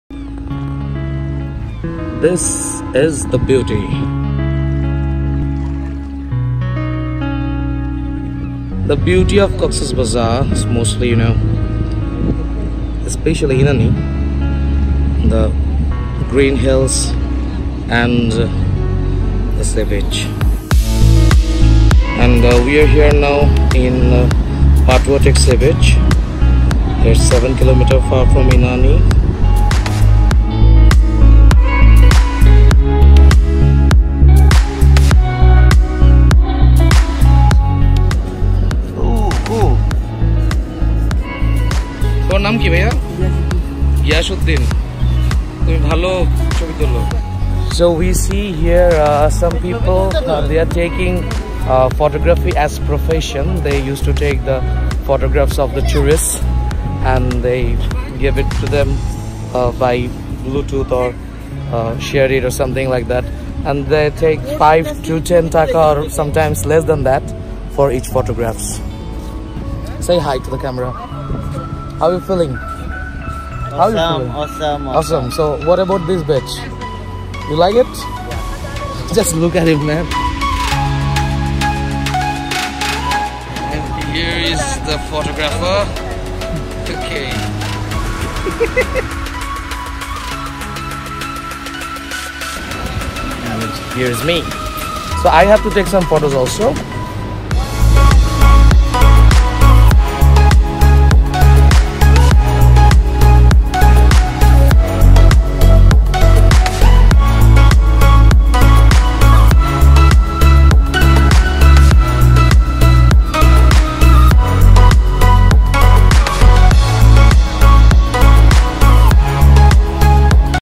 COX'S BAZAR, কক্সবাজার, PATUATEK sea beach
Patuatek is a sandy beach with coral reef at Cox'sbazar, it is 8 KM far from Inani. i found it as the most beautiful beach of Cox'sbazar, Bangladesh.